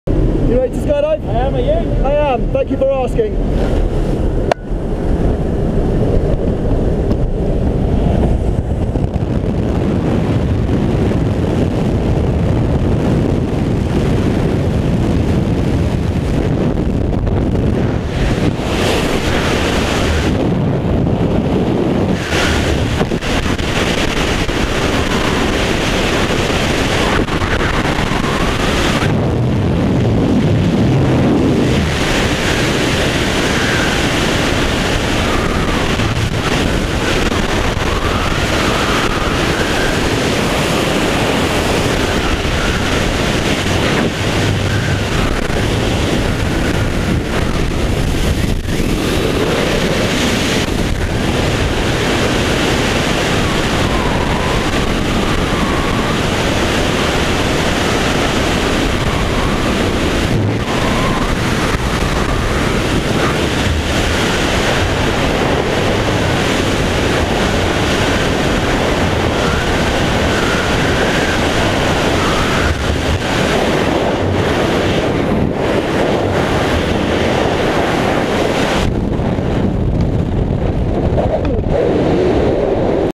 2016-08-14 AFF L6 Matt Hurford
Matt goes Skydiving. This is his AFF Level 6 jump. Back loop and a track is the plan and he had time left over to chuck in a cheeky 360 turn too. Awesome job!